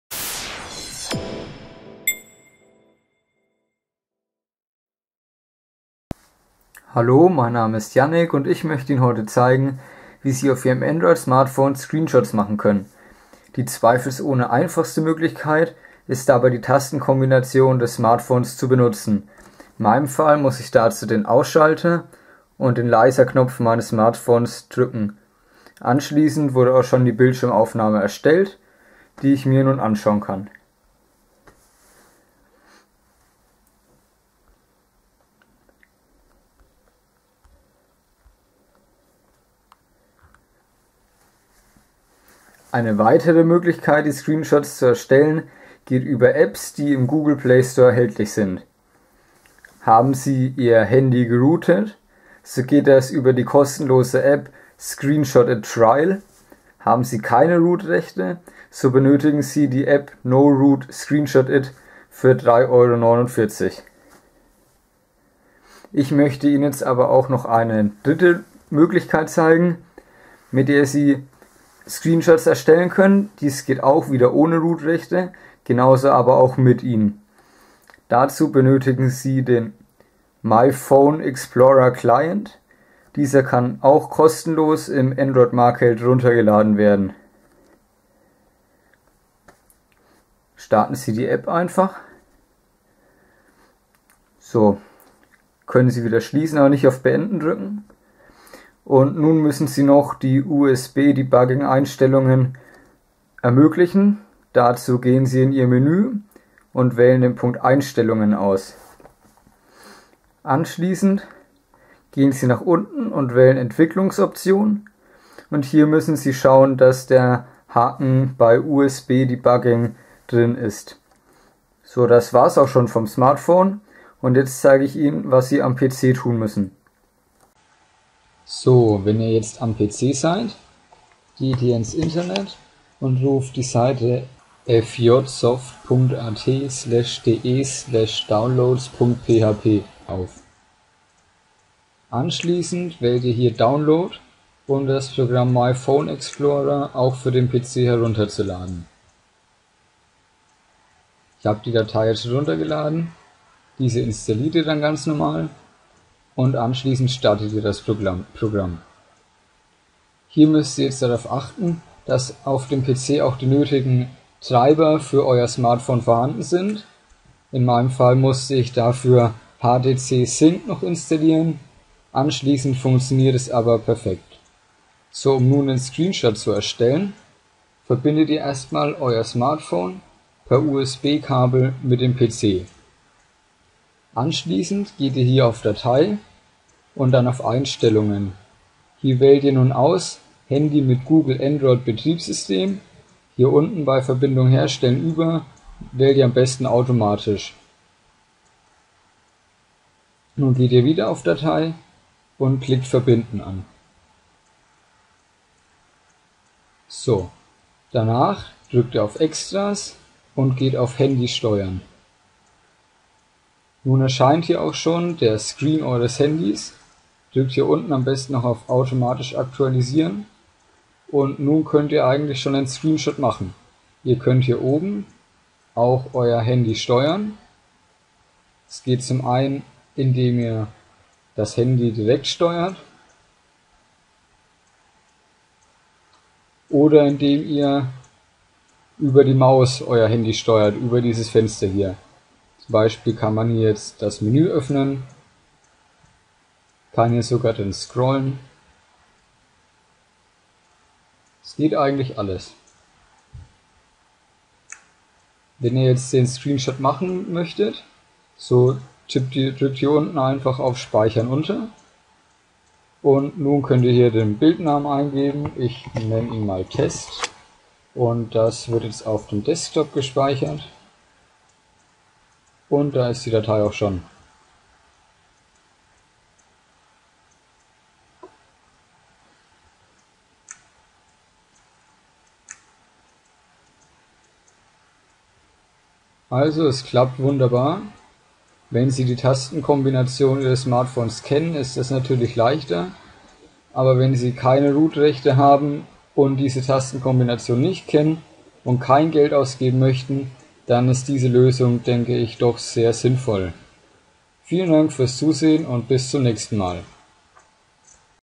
Mit Android kostenlos Screenshots ohne Root-Rechte erstellen. Supportnet Tipp
Erfahren Sie in diesem Video wie man mit einer kostenlose App Screenshots vom Android Smartphone machen kann und das ohne Root-Rechte.

Screenshots auf einem Android Gerät zu erstellen ist etwas knifflig. Da Bilder aber oft mehr sagen als tausend Worte, ist die Funktion doch sehr wichtig. Wie Sie mit Ihrem Android Smartphones Screenshots unabhängig von Root Rechten machen können, werden wir Ihnen in diesem Artikel zeigen.